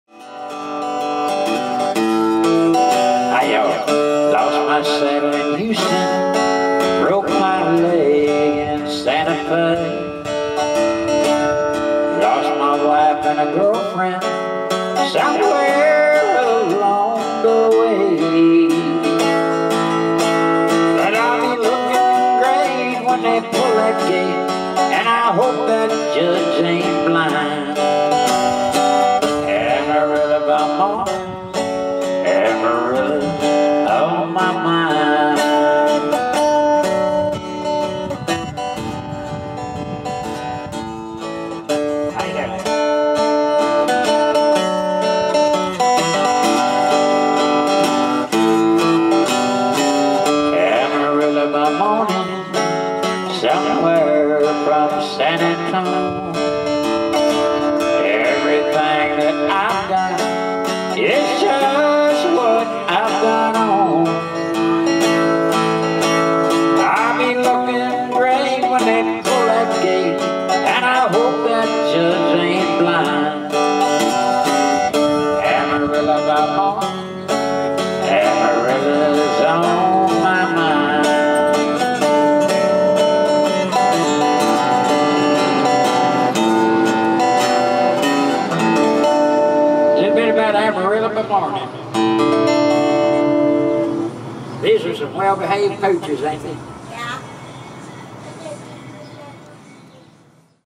Fort Worth Stockyard Blues | Sony a6300 (example 4K-UHD footage)
Shot handheld on the Sony a6300 E-mount camera in S-Log3 Cine gamma with native 35mm f/1.8 crop lens and Sony ECM-XYST1M stereo microphone (forgot the dead cat!), using Clear Image Zoom (2.0x) and auto-focus continuously. Graded with FilmConvert, mastered in 4K-UHD.

Fun tip:  look for the "pooches," they're worth the wait!